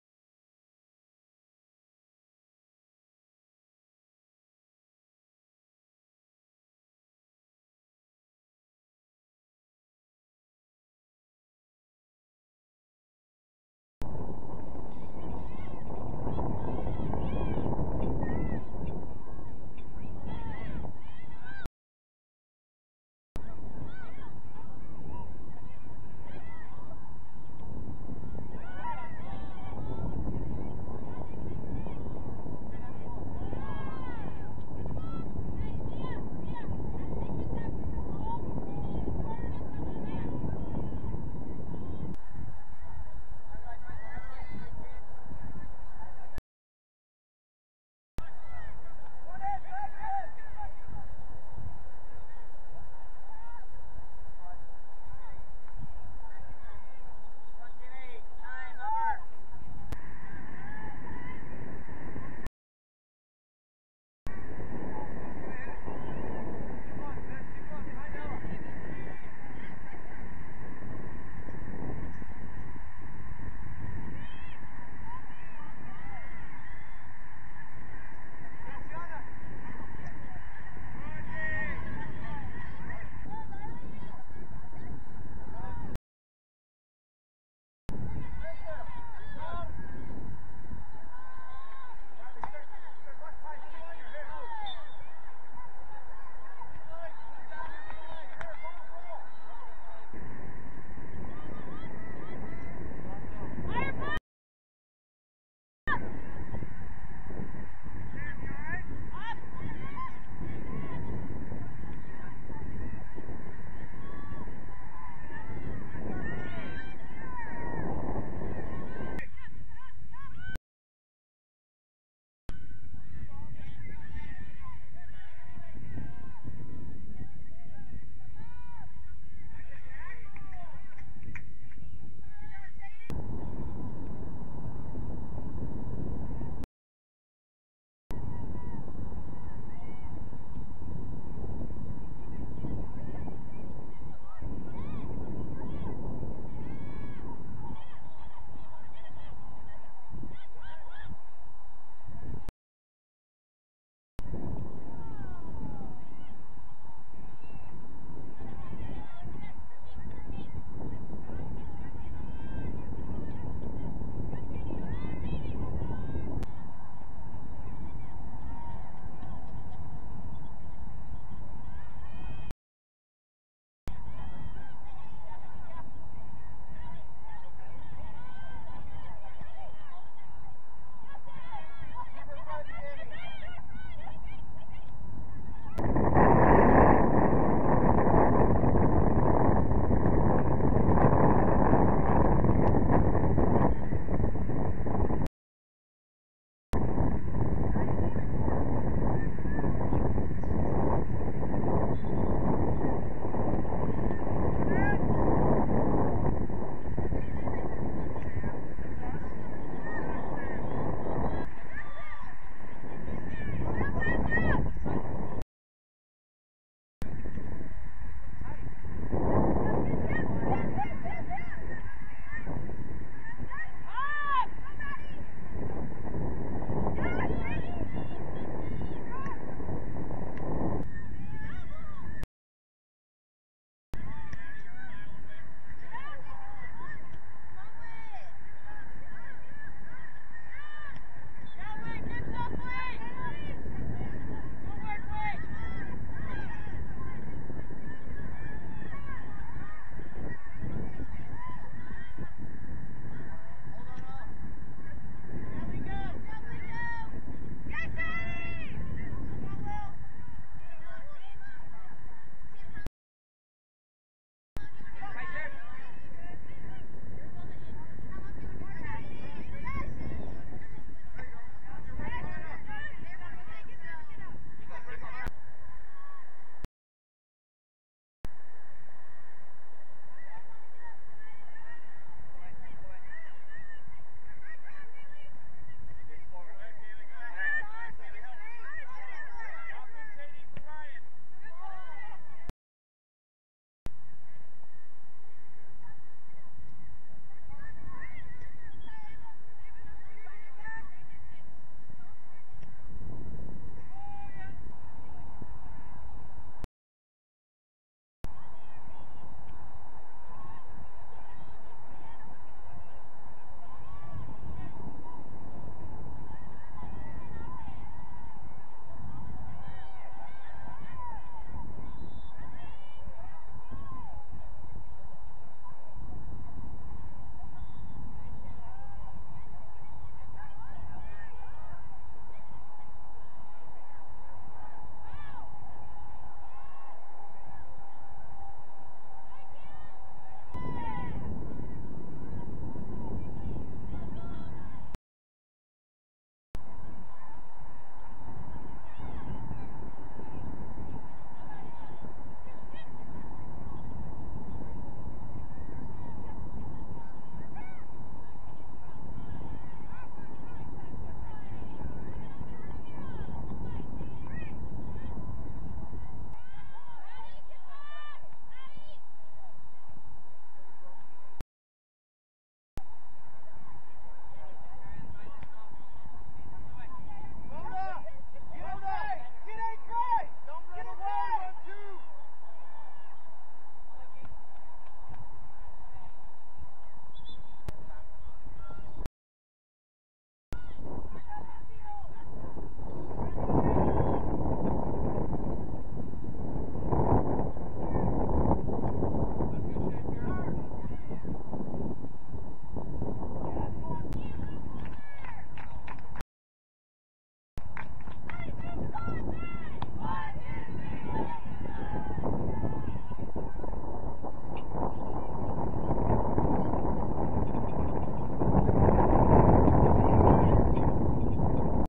Cheney Carey- 2024 Racing Louisville ECNL Spring
This is my latest highlights from Spring 2022 season at Racing Louisville.